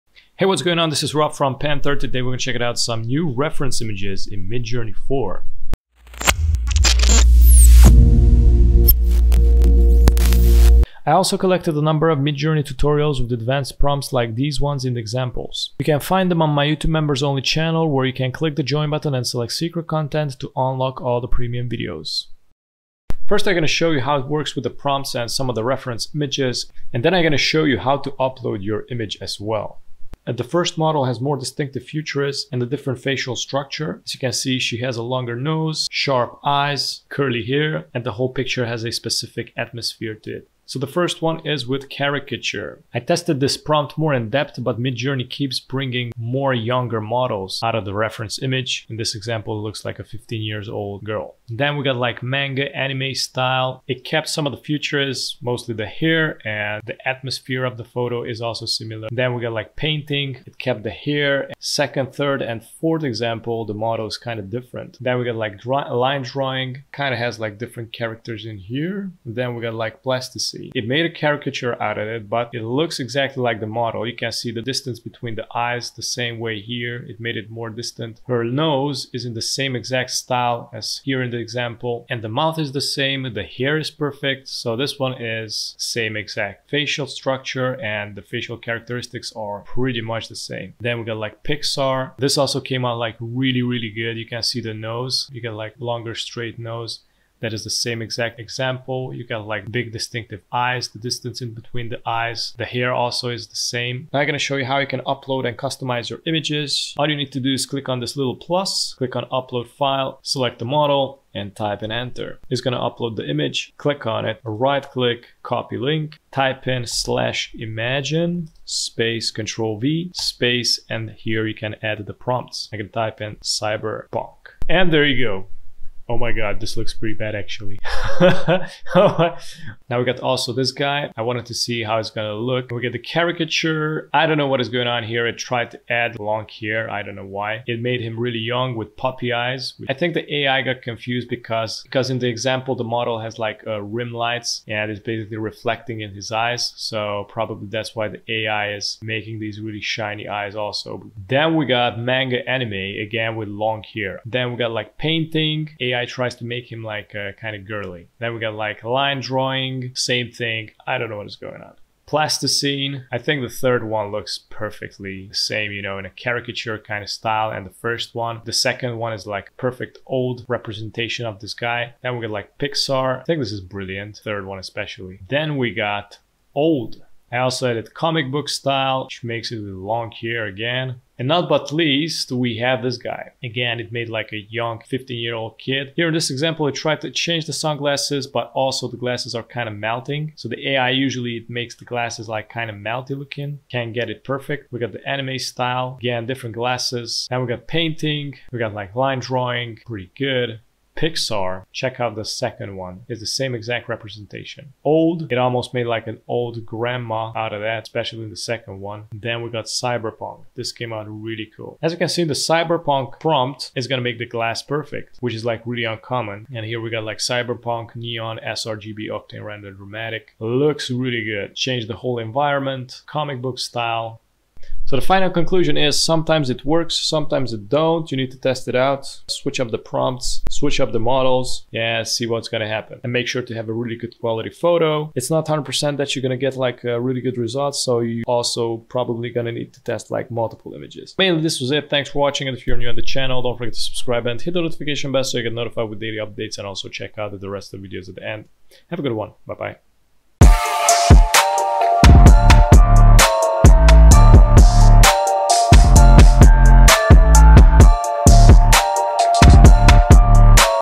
How to use Reference Images in MidJourney V4
MidJourney 4 seems like it came back with stronger updates regarding when uploading your images for reference and now it gives you even better results rather then Midjourney 3 did before. It's still a work in progress but it's getting better and better month after month.

0:00 Intro
0:10 Promo
0:23 Midjourney 4 setup
0:31 Reference image 01
1:44 Upload your image in Midjourney
2:09 Reference image 02
3:01 Reference image 03
3:45 Final conclusion
4:11 Outro